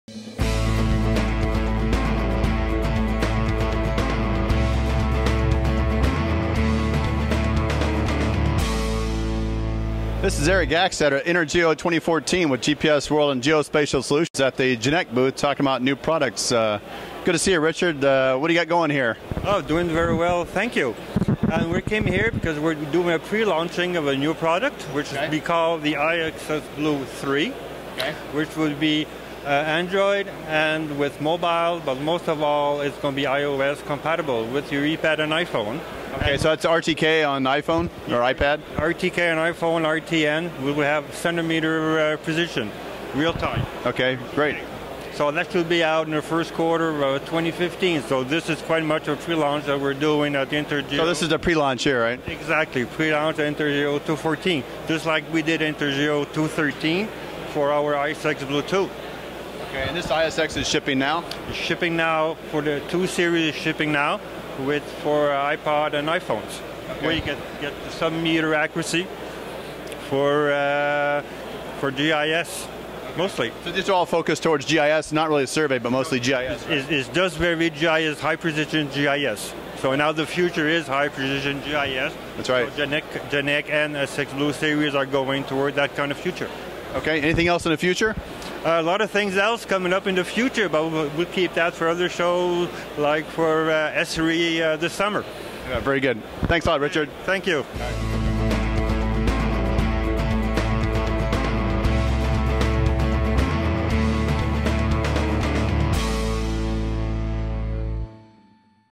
InterGeo 2014: Geneq Inc.
Richard Desrochers discusses Geneq Inc.'s pre-launch of its SXBlue III GNSS RTK Receiver while at the 2014 InterGeo Conference and Trade Fair held October 7-9 in Berlin.